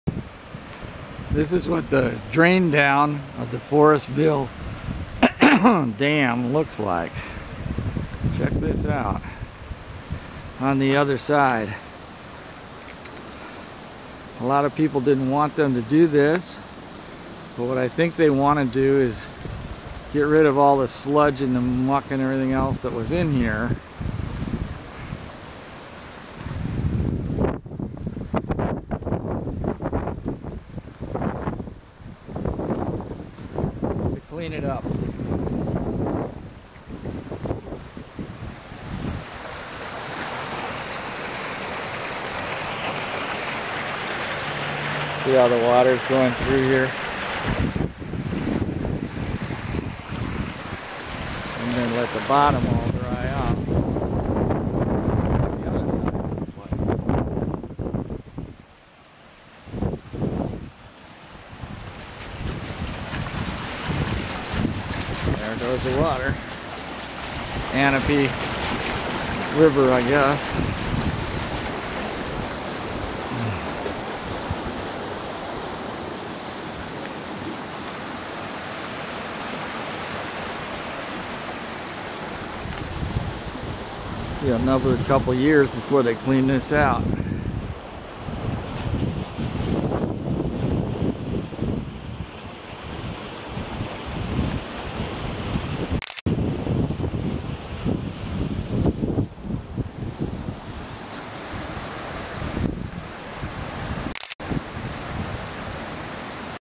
Forestville dam drain down
This is what the millpond looks like without water in it. The water level is normally up to the top of the "teeth" of the dam. I think the intention is to clean out the muck and debris to clean up the pond for future users.